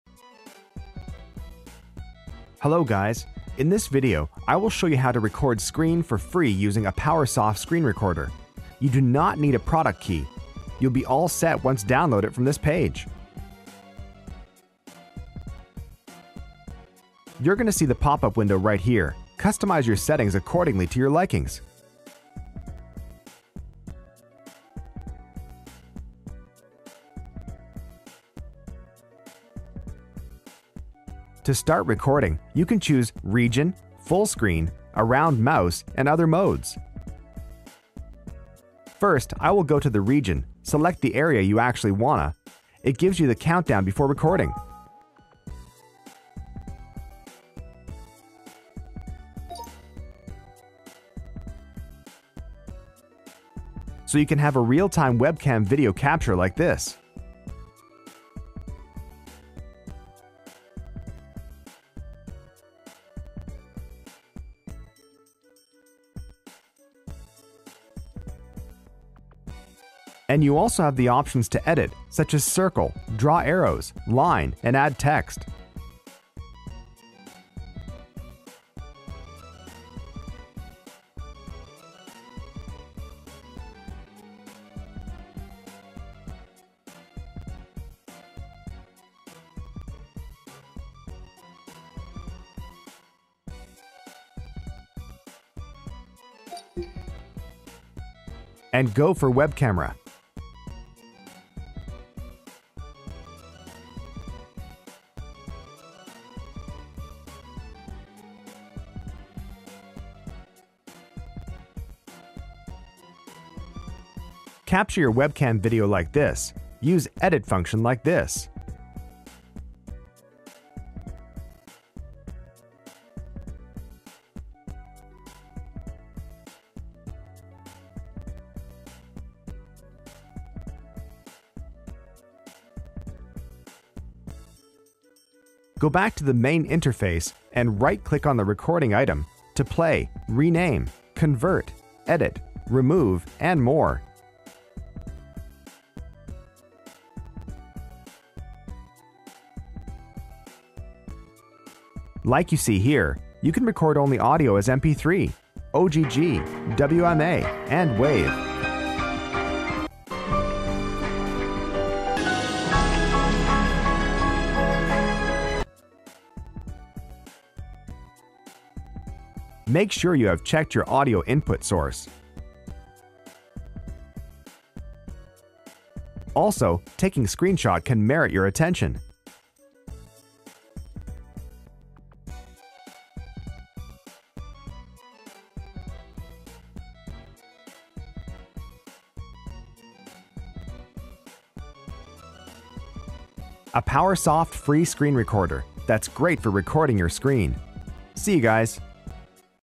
Best free desktop screen recorder - Apowersoft Screen Recorder
Apowersoft Free Screen Recorder can record anything that happens on your windows desktop, along with audio or commentary.You are able to record the entire screen,webcam,or part of your screen.